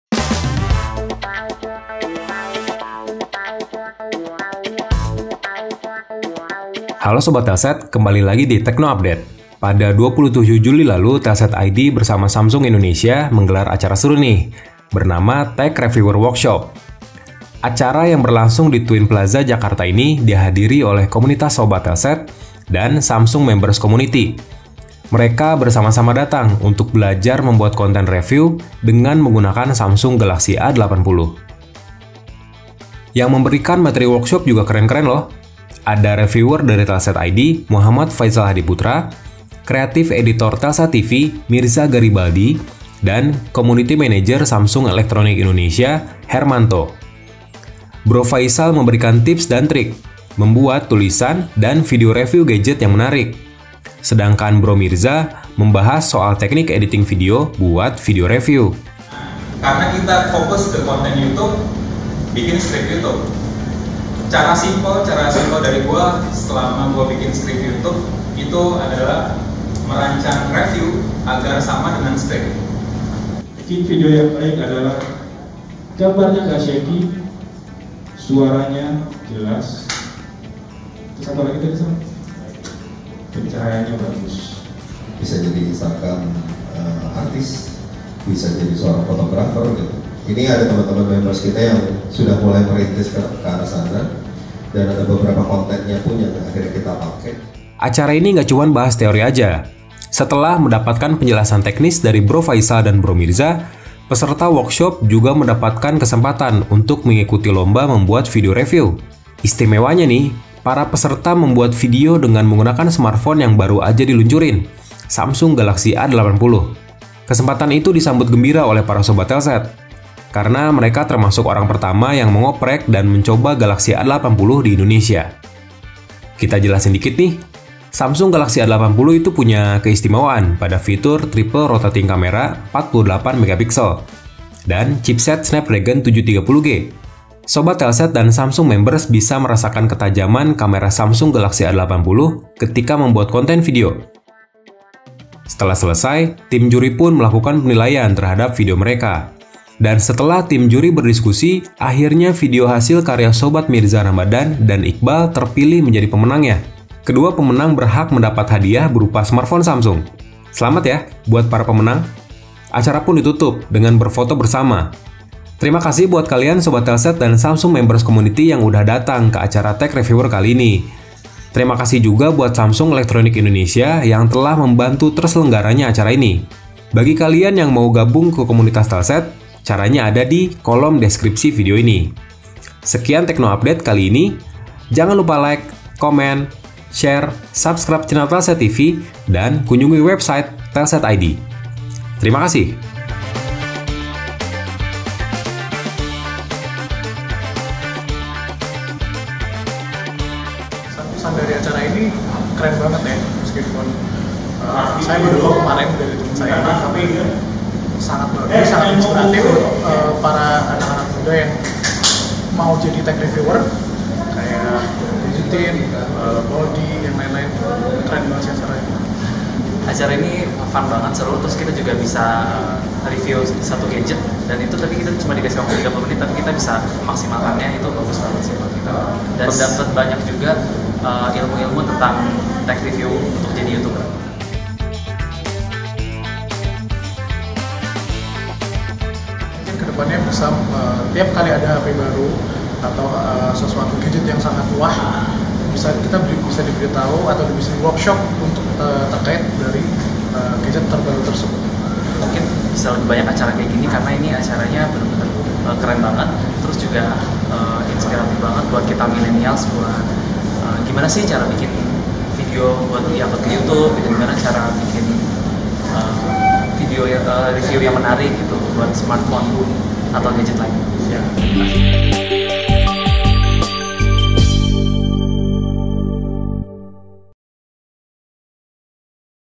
TECH REVIEWER WORKSHOP: Seru-seruan Ngoprek Samsung Galaxy A80
27 Juli lalu, Telset.id bareng Samsung Electronic Indonesia mengadakan acara bertajuk "Tech Reviewer Workshop". Acara ini dihadiri oleh Komunitas Sobat Telset dan Samsung Members Community.

Bersama dengan reviewer dari Telset.id, Muhammad Faisal Hadi Putra, kemudian Creative Editor Telset TV, Mirza Garibaldi, dan Community Manager Samsung Electronic Indonesia, Hermanto, para peserta acara sama-sama belajar membuat konten review, mengabadikannya melalui video, melakukan editing, hingga upload ke YouTube menggunakan Samsung Galaxy A80.

DAFTAR KOMUNITAS TELSET
Kirim identitas Kamu
(Nama, Alamat, Alamat email) ke;